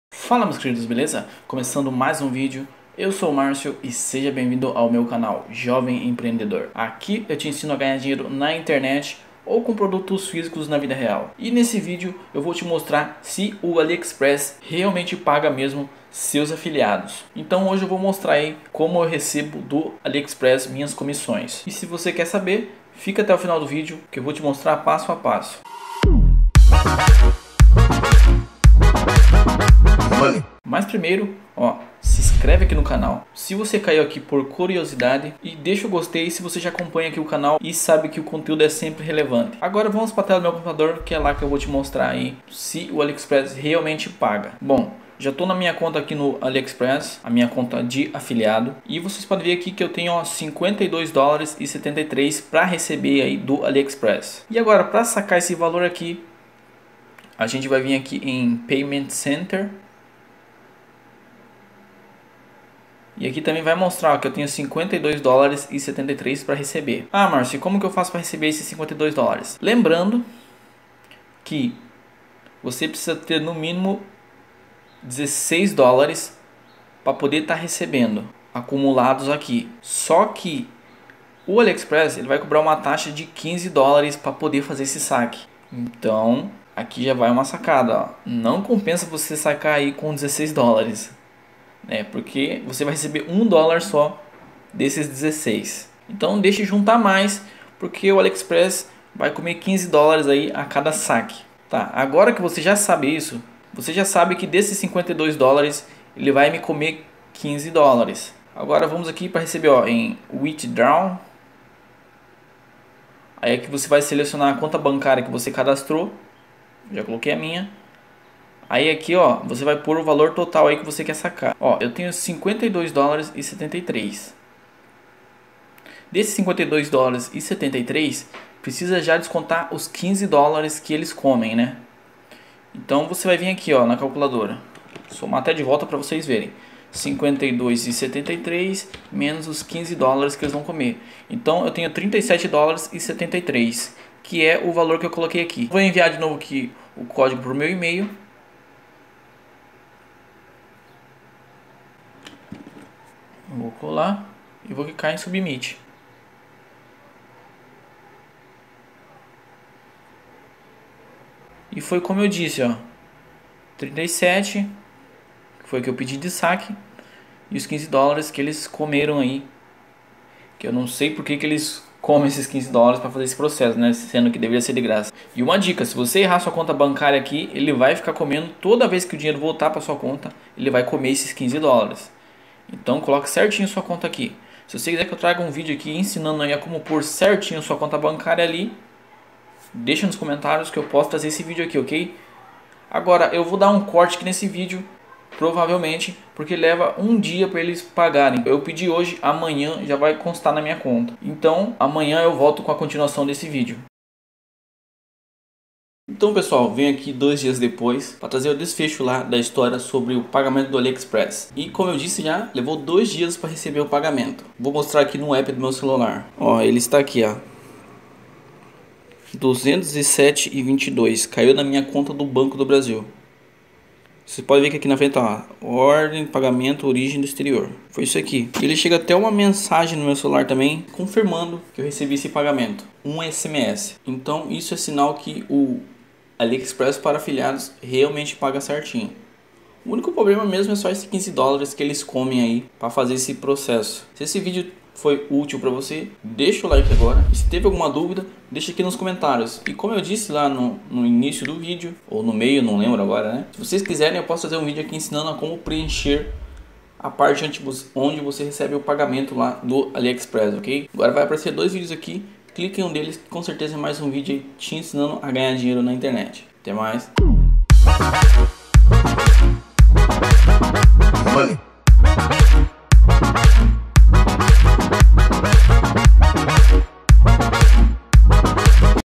AFILIADO ALIEXPRESS PAGA MESMO? Verdade Revelada! | APRENDA COMO SACAR NO ALIEXPRESS COMO AFILIADO
Nesse vídeo vou mostrar que ganhar dinheiro no Aliexpress é possivel sim, e vou provar que o Afiliados Aliexpress realmente paga! Aproveitando o vídeo também ensino como sacar no aliexpress como afiliado.

Se ainda não sabe o que é o programa de afiliado aliexpress, ele basicamente é uma plataforma que te paga para divulgar os produtos do aliexpress, e a cada venda feita através do seu link, você ganhar comissão em dólar.

Se caiu nesse vídeo com a intenção de ouvir sobre o suameta, clubesuameta ou rodrigovitorino. Desculpa! mas pode fechar o vídeo.

Quer saber como ser afiliado aliexpress também ? Vou deixar o link fixado nos comentários desse vídeo